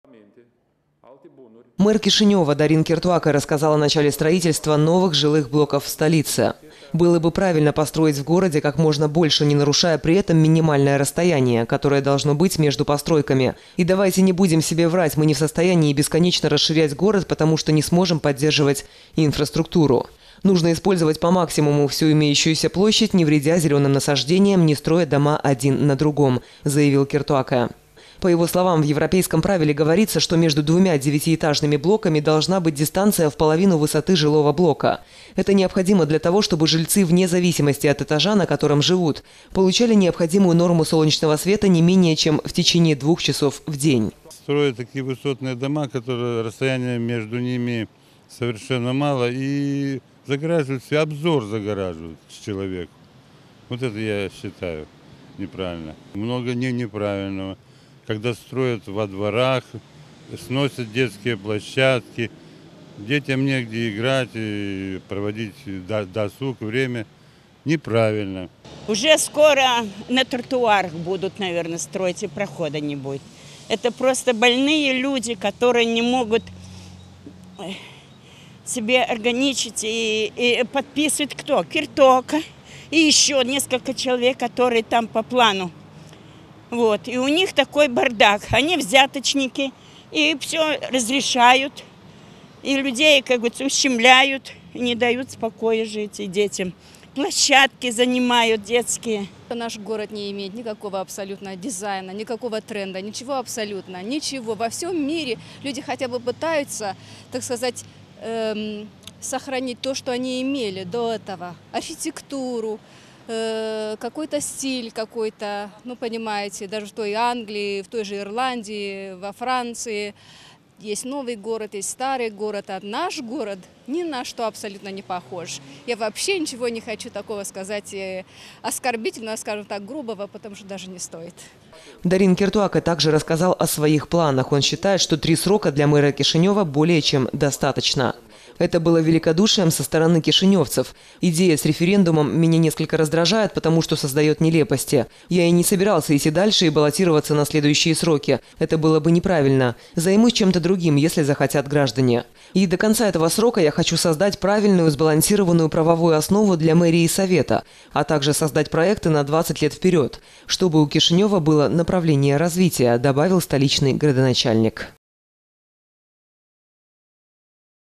Проекты от мэра на 20 лет вперед
Мэр Кишинева Дорин Киртоакэ рассказал о начале строительства новых жилых блоков в столице.
«Было бы правильно построить в городе как можно больше, не нарушая при этом минимальное расстояние, которое должно быть между постройками. И давайте не будем себе врать, мы не в состоянии бесконечно расширять город, потому что не сможем поддерживать инфраструктуру. Нужно использовать по максимуму всю имеющуюся площадь, не вредя зеленым насаждениям, не строя дома один на другом», - заявил Киртоакэ.